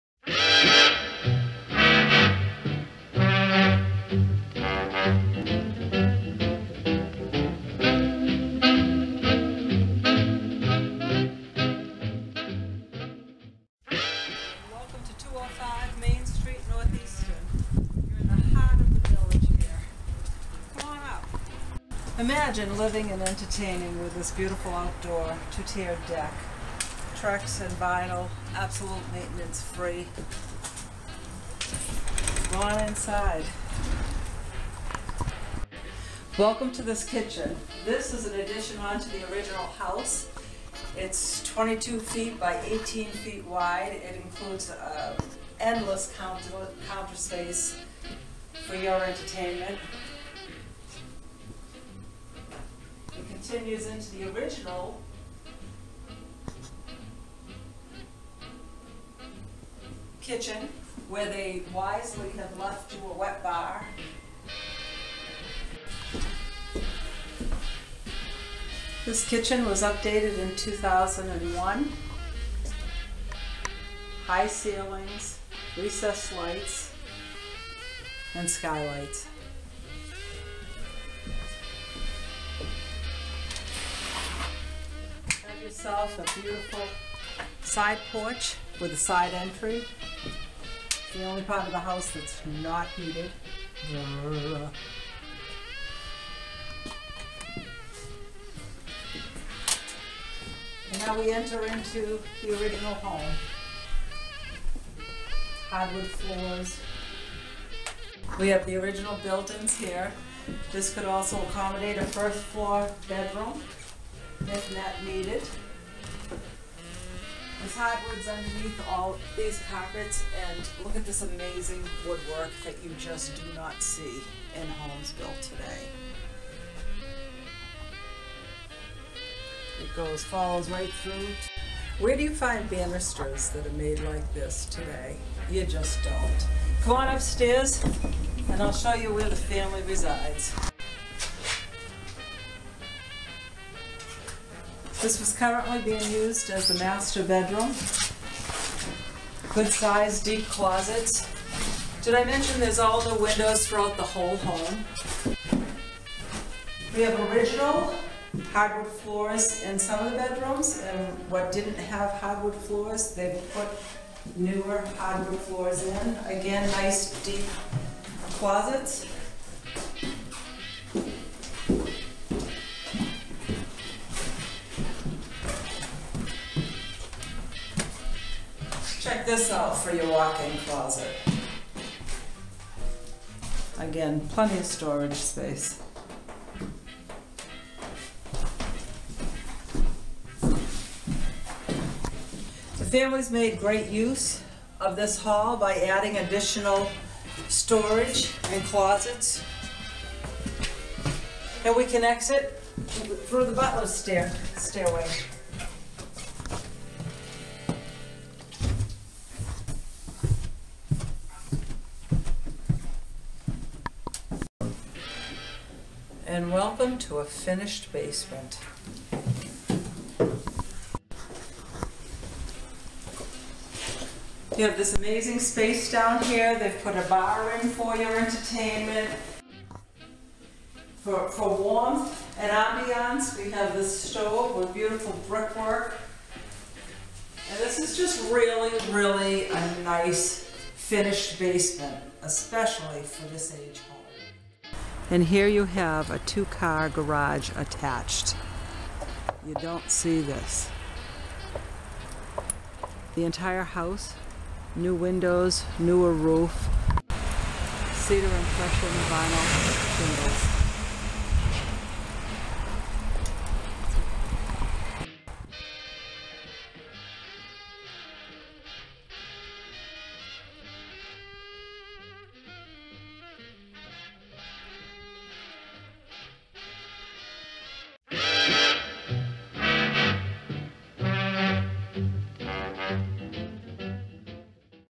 205 Main St, Easton Village, MA
A private oasis located in the heart of North Easton Village! This Classic New England Victorian style home offers loads of old world charm in immaculate and updated condition. The SUPER SIZED Kitchen/ Dining room, with incredible mill work and high ceilings hardwood floors on both levels. This home could accommodate a 1st floor bedroom. Enjoy peaceful evenings and weekends on this lovely 2 tier trex 22"x24' deck overlooking the back yard, perfect for entertaining or relaxing with a good book. Two separate stair cases, lead to three spacious BRs with W/I closets. Two car attached garage. This property has not been available in over 80 years. A must see!!